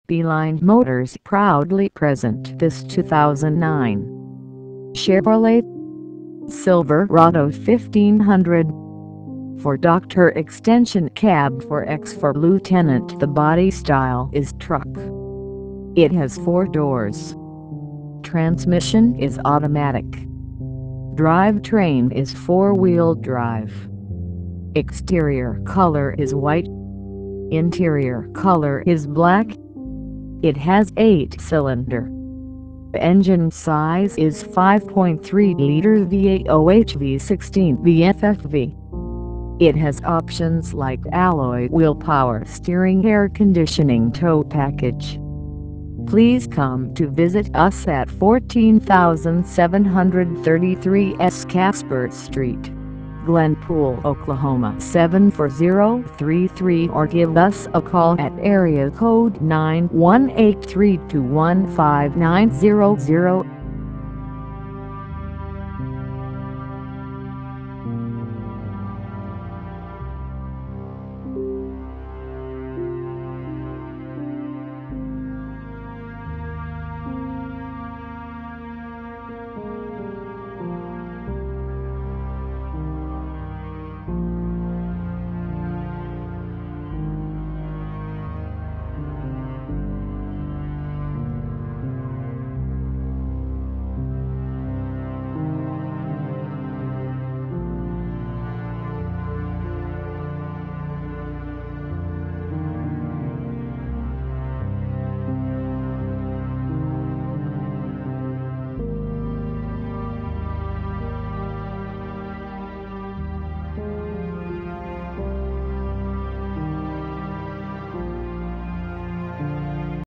2009 Chevrolet Silverado 1500 $17950 by B-Line Motors
2009 Chevrolet Silverado 1500 , 4dr Ext Cab 4X4 LT, $17950, 130939 Miles , B-Line Motors, check more inventory from this dealership 14733 S. Casper Street Glenpool OK 74033 , , Automatic, 4 wheel drive, Ext Color: White, Int Color:Black, Body Style: Truck, Engine Size: 5.3L V8 OHV 16V FFV (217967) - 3/14/2015 4:14:44 PM - 3/13/2015 3:09:39 PM.